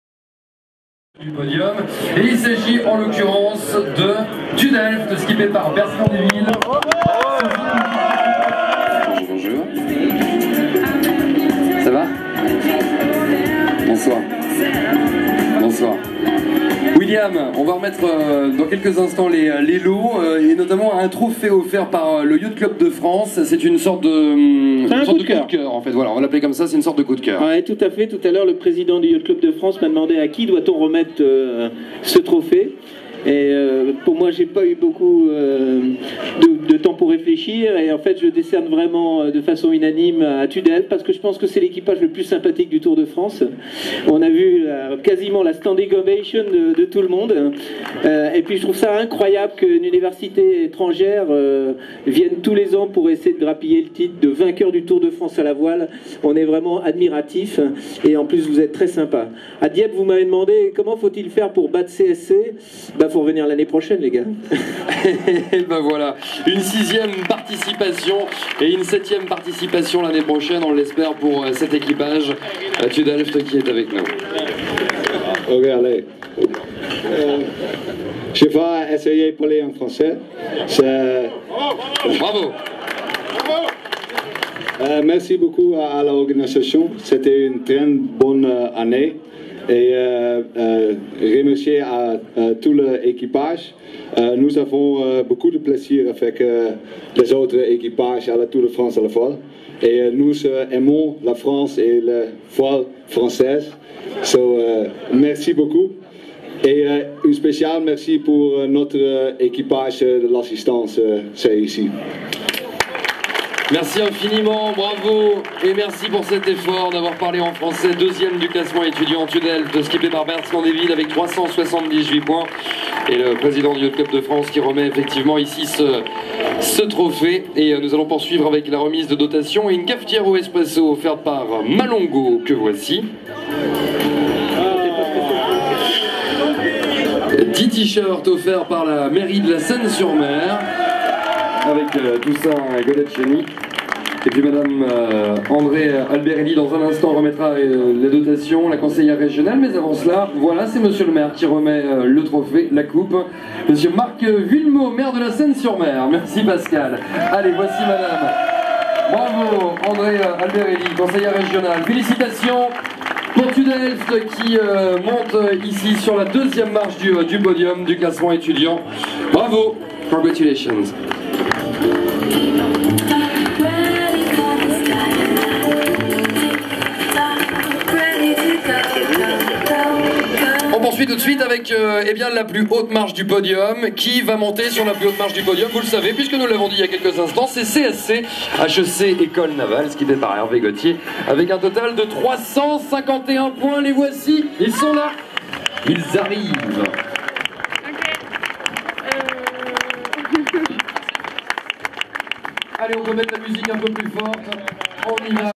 prijsuitrijking
Team Delft Challenge op het podium voor de 2e plaats in het studentenklassement!